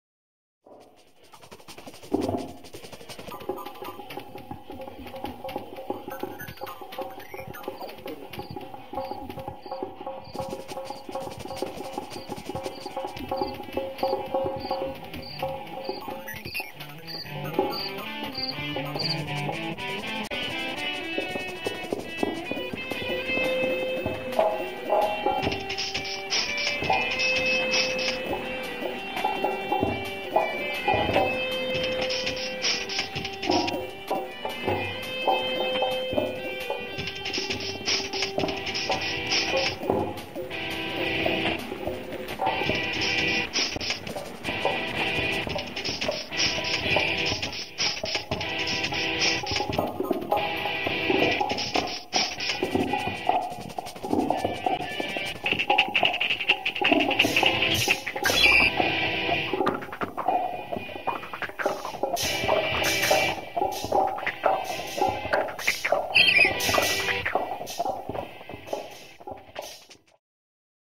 Exquisite Haiku
Exquisite Haiku is a synthesis of the Surrealist game "Exquisite Corpse" and the traditional Japanese poetry form Haiku. The hakku used here were generated by "The Genuine Haiku Generator"

A DC Spensley Artwork
Scripts by Solo Mornington